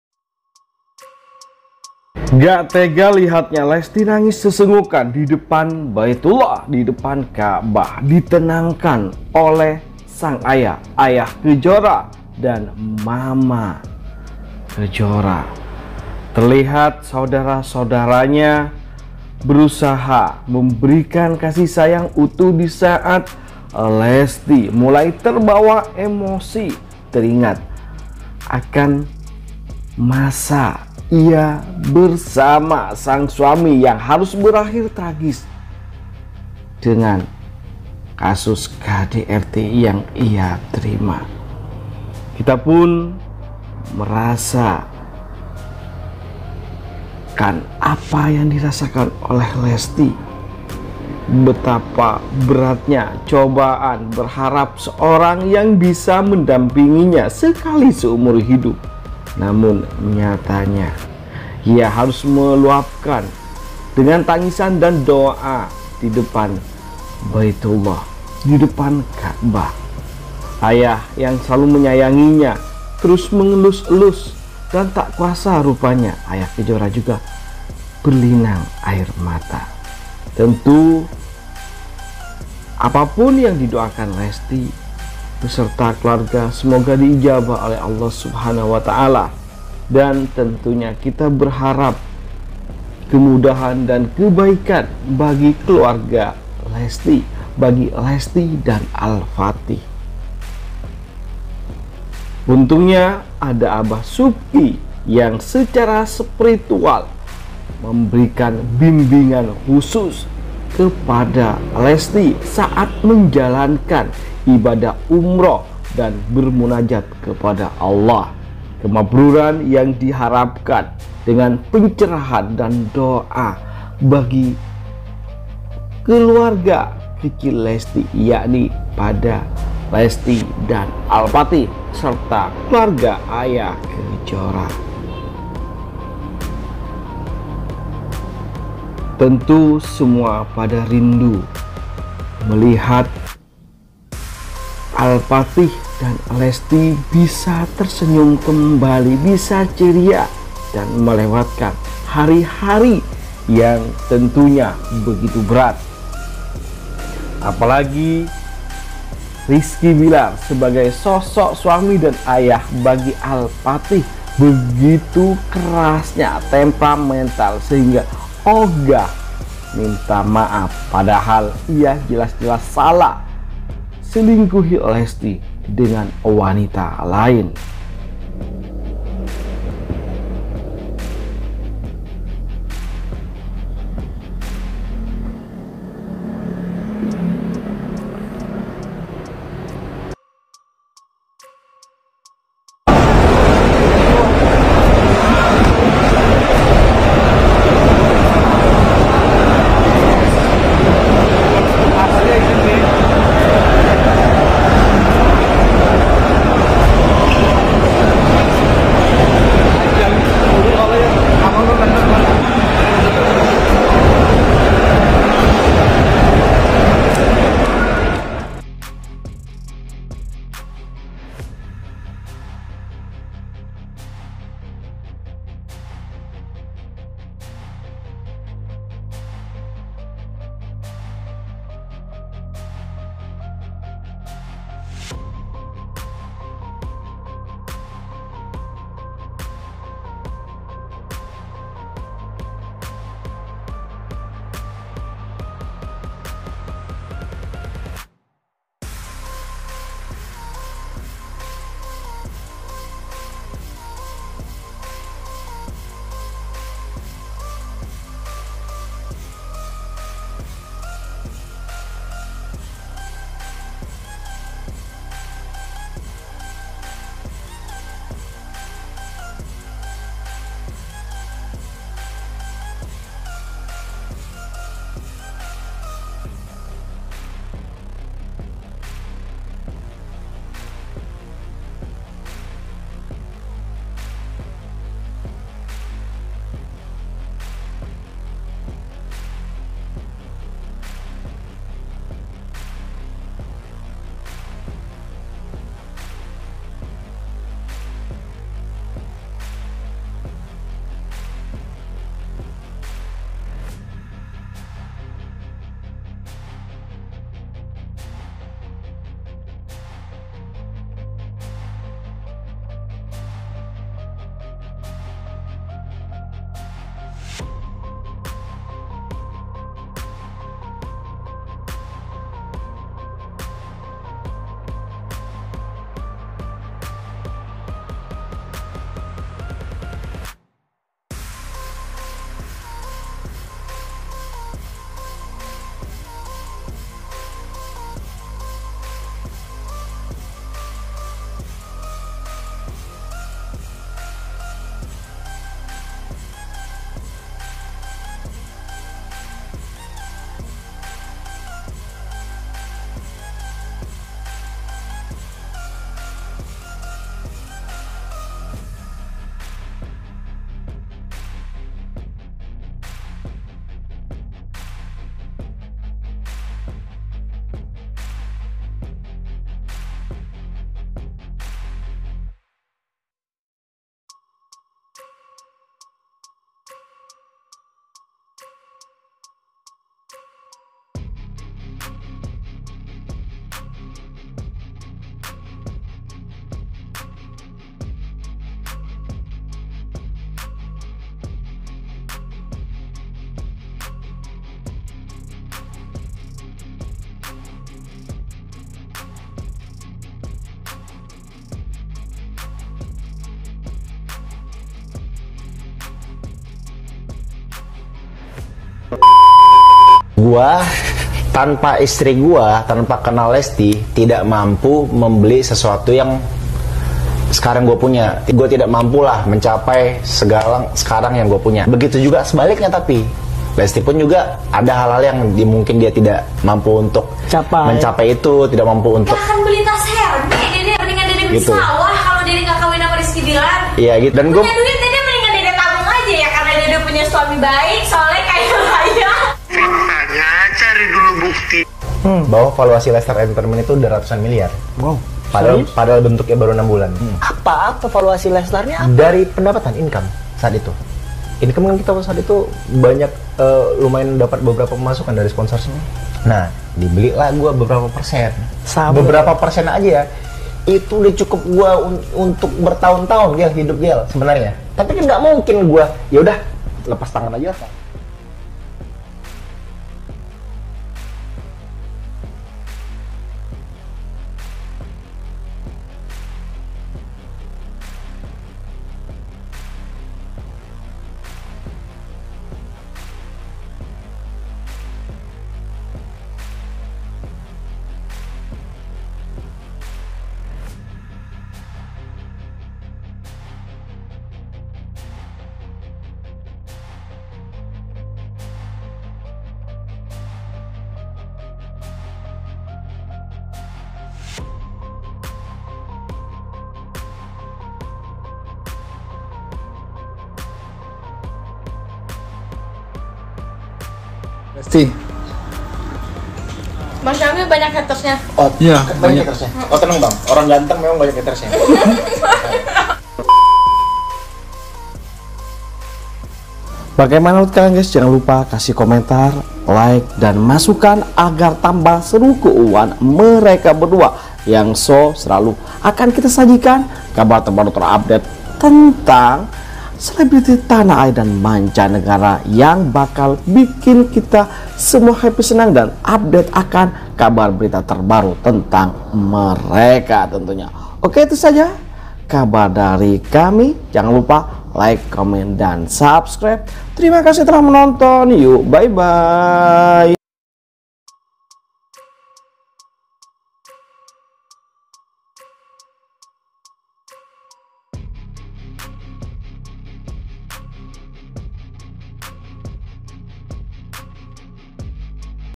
Detik2 Lesti NANGiS Histeris Dpn Ka'bah, Ayah Kejora TENANGKAN Putrinya❗️
Jangan Lupa: _
Thankz!!..